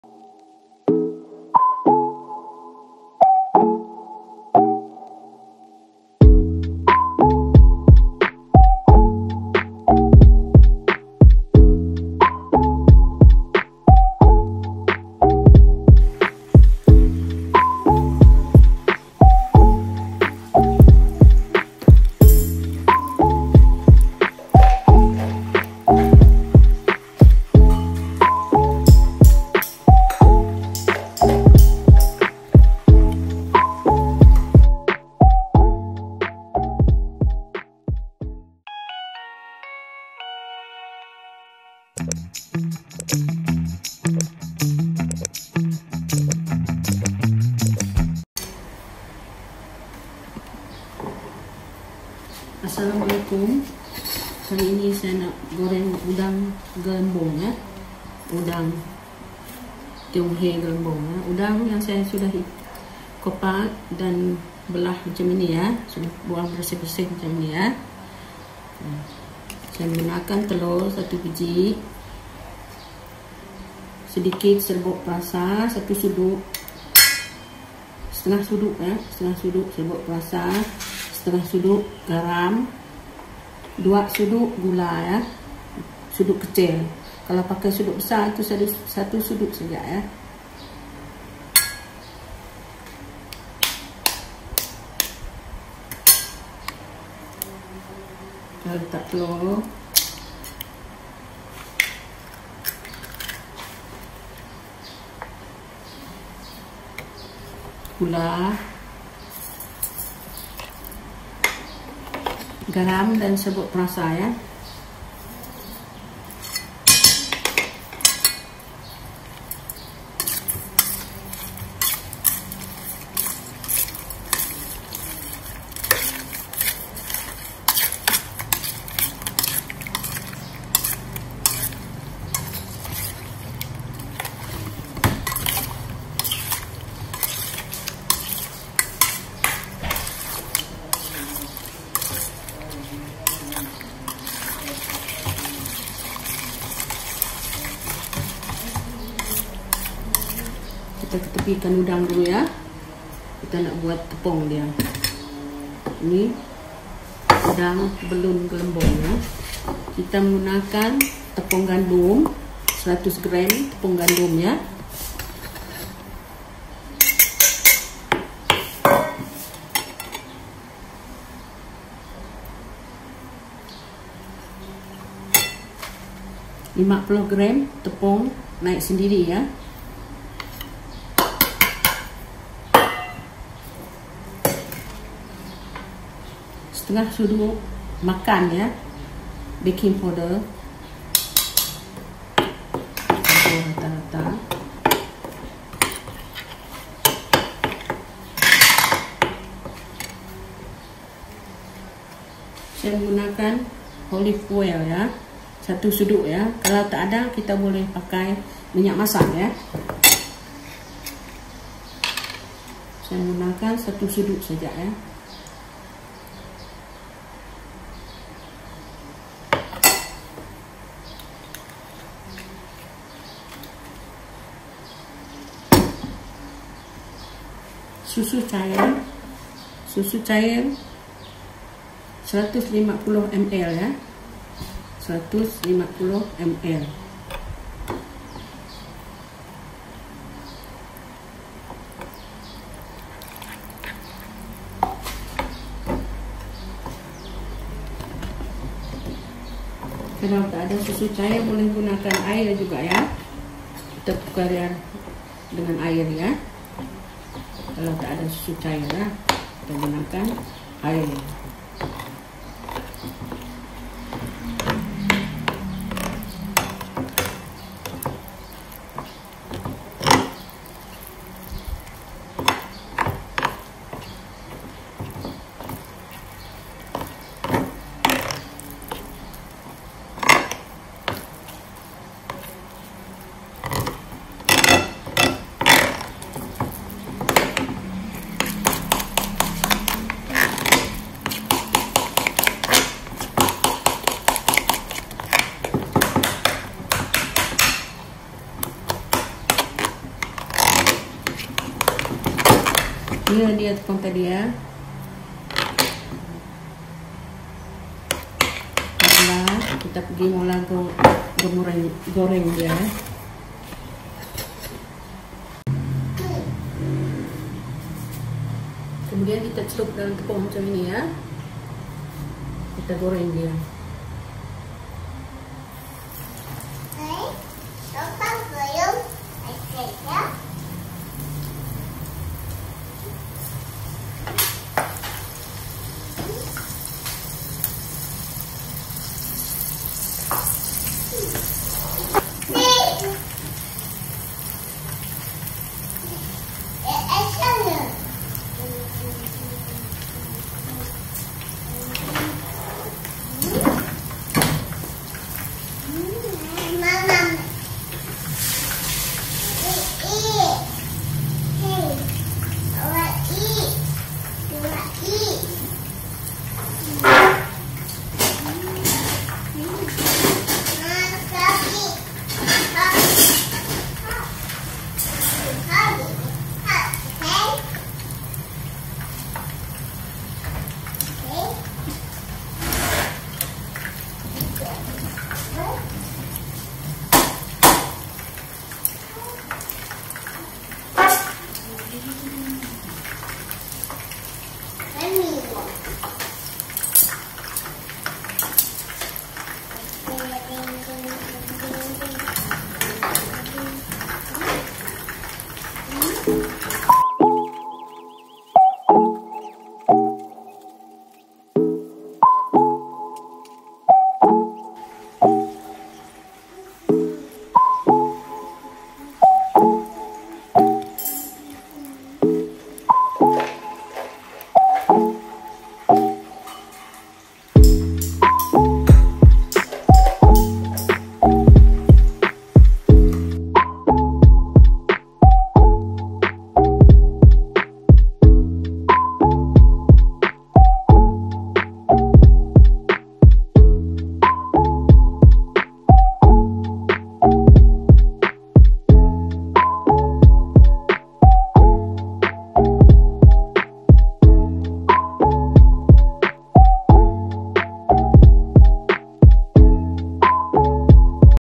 UDANG CELUP TEPUNG | UDANG GORENG TEPUNG ALA THAI
​@DapurMamaAhLek goreng udang goreng tepung ala Thai atau
resepiudang ini cicah dengan sos Thai memang enak sekali.

⊱⋅ R E S E P I R A M A D A N J U A D A H B E R B U K A P U A S A ⋅⊰